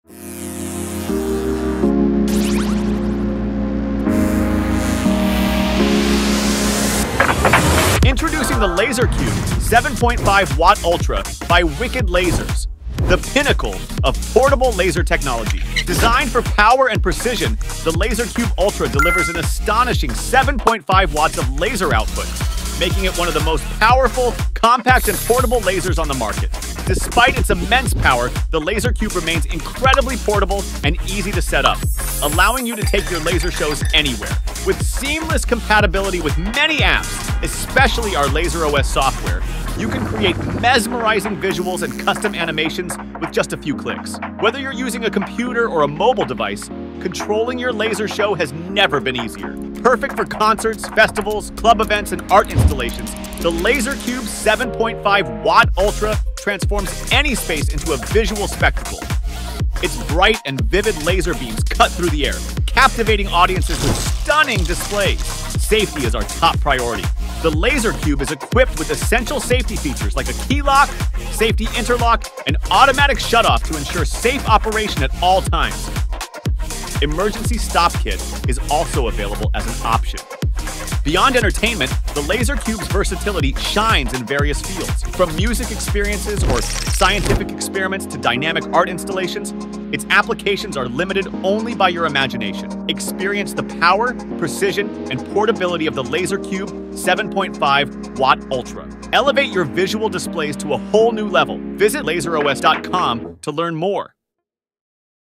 Portable Laser Show Projector: LaserCube 7.5W Ultra by Wicked Lasers 🔥
Hello Laser Fam!

Here is another breathtaking shot of our bestselling laser show projector: LaserCube 7.5W Ultra 😍

🔥 7500mW of pure laser power
🔥 Upgraded AT-40S scanning system
🔥 Very quiet and efficient cooling system
🔥 just 3,3kg weight